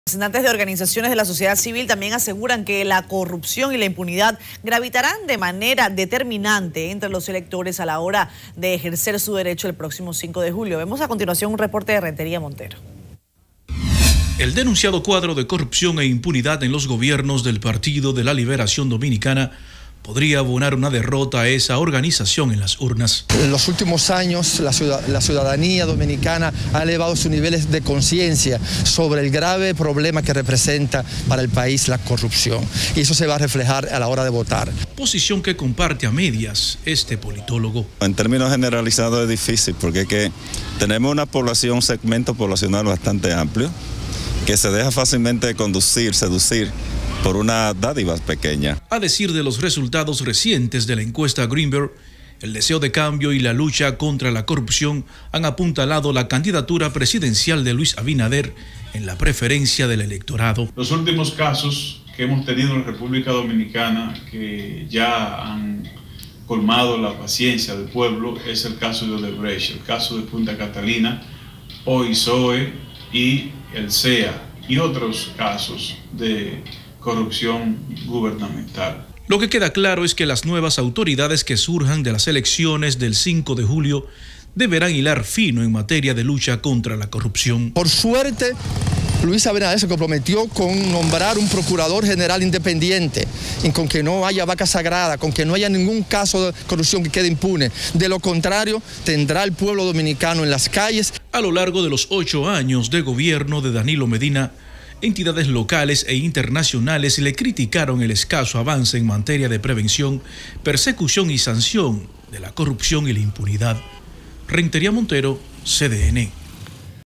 Temas corrupción e impunidad podrían impulsar voto de castigo contra partido de Gobierno
Representantes de organizaciones de la sociedad civil aseguran que el tema de la corrupción y la impunidad gravitarán de manera determinante entre los electores a la hora de ejercer su derecho al voto en los comicios del próximo 5 de julio.

El denunciado cuadro de corrupción e impunidad en los gobiernos del Partido de la Liberación Dominicana podría abonar una derrota de esa organización en las urnas.

A decir de los resultados recientes de la encuesta Greenberg, el deseo de cambio y la lucha contra la corrupción han apuntalado la candidatura presidencial de Luis Abinader en la preferencia del electorado.